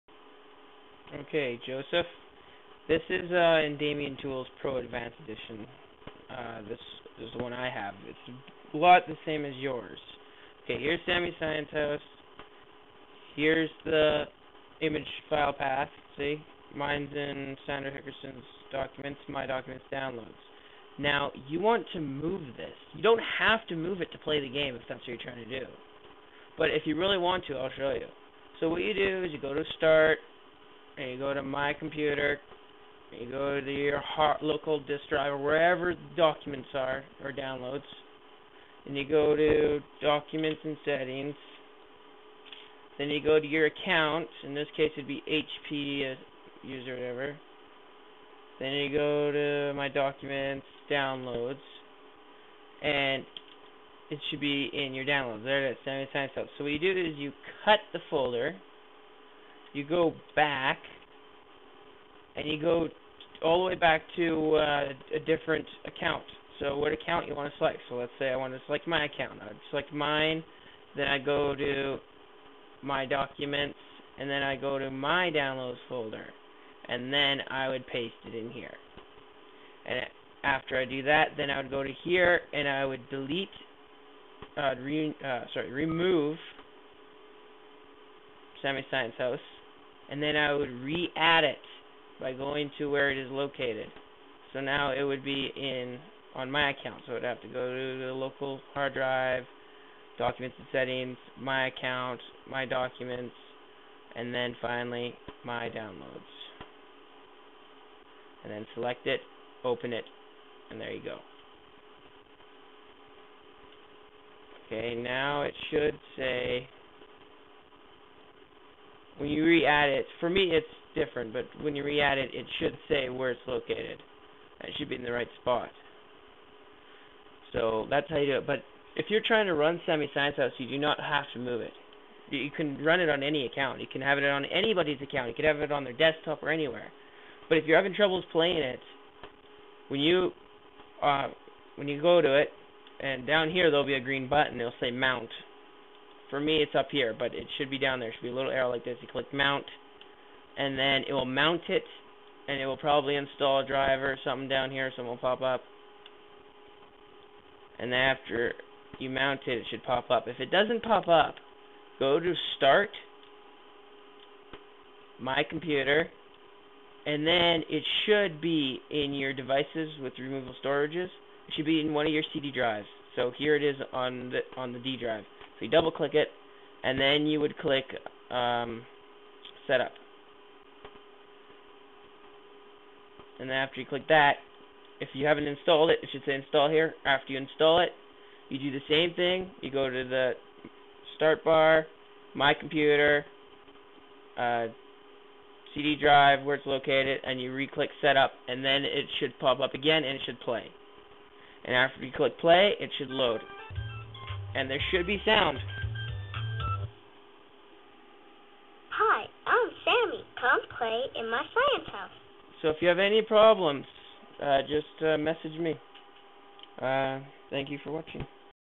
How to move Sammy's Science House to a different user (Windows XP)
This will show you hot to move Sammy's Science House.iso to a diffrent users account WITHOUT logging off